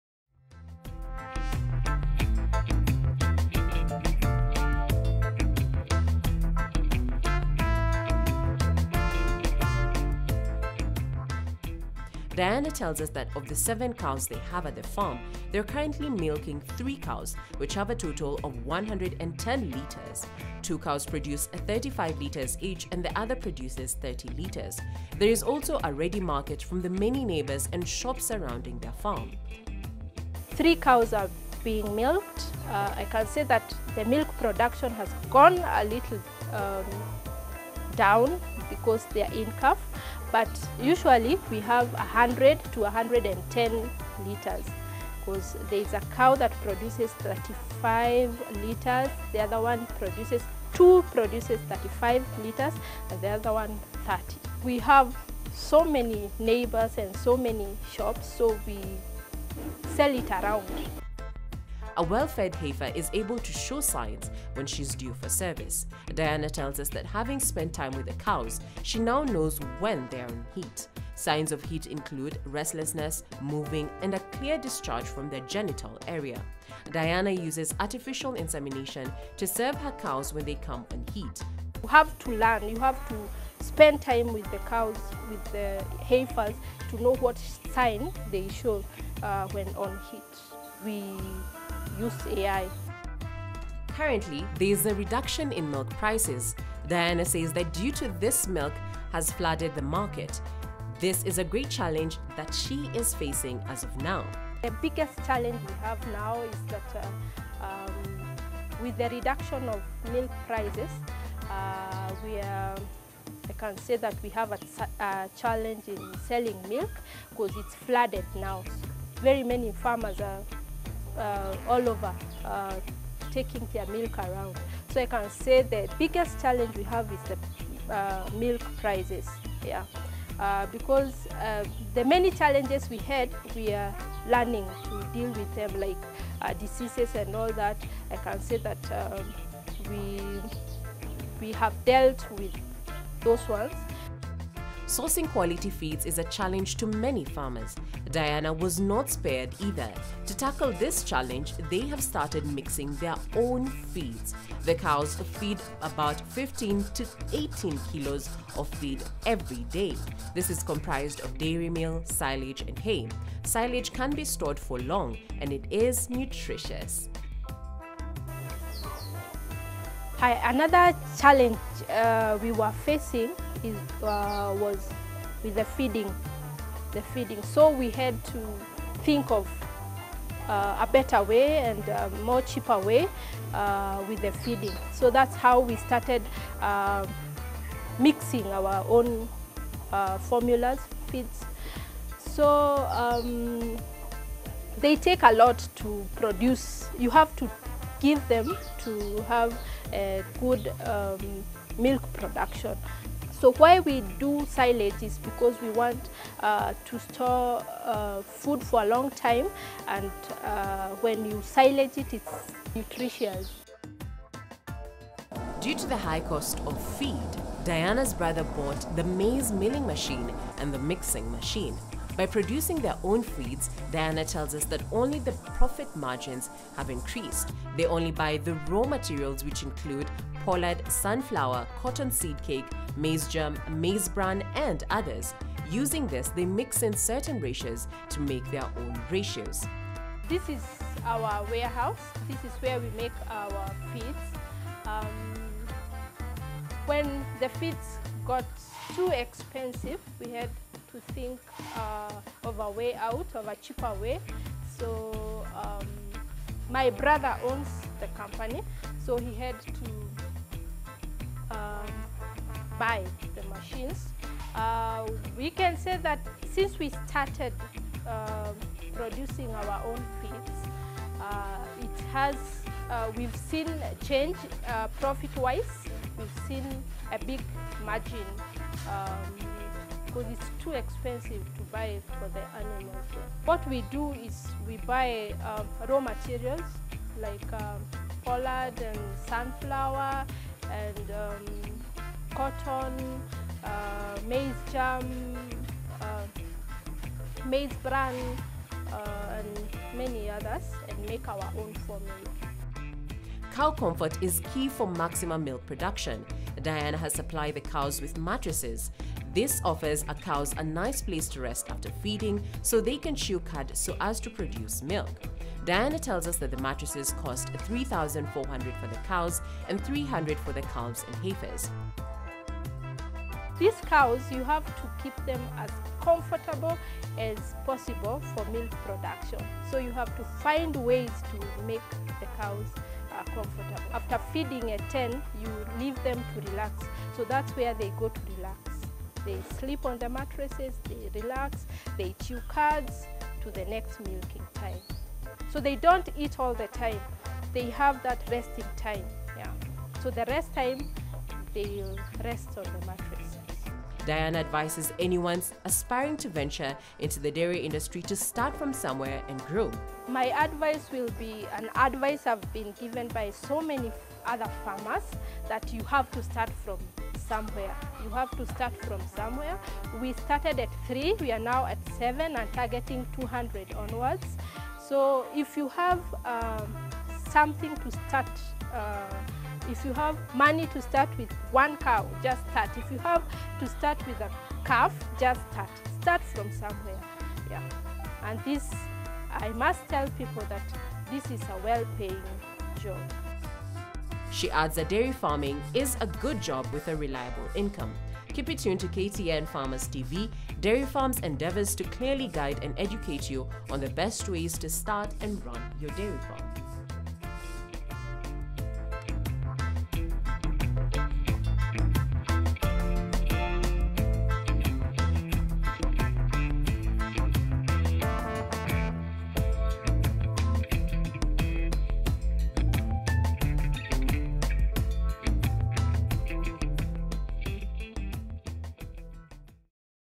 Ditched Photocopy Business for Dairy farming - Part 2
Diana a young vibrant farmer motivated by her parents, quits her employment from a photocopying business in Nairobi and dives into dairy farming.

SMS: 22071